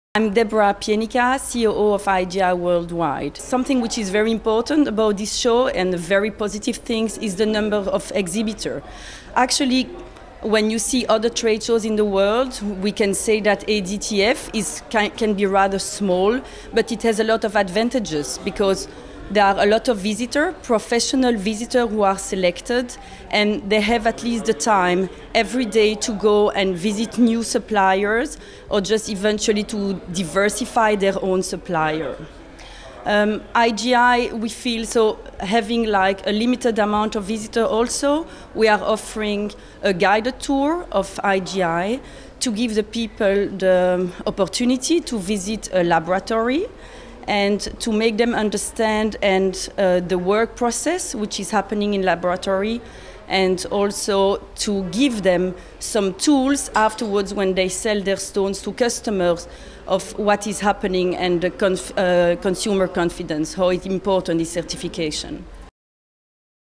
Deborah Pienica
Antwerp Diamond Trade Fair interview Deborah Pienica